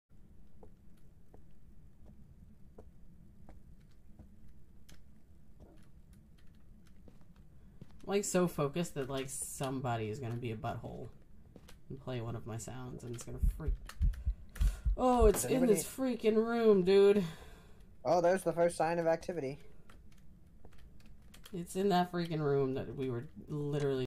Sath and I find the first signs of Activity from the ghost
It took us forever to find this Ghost!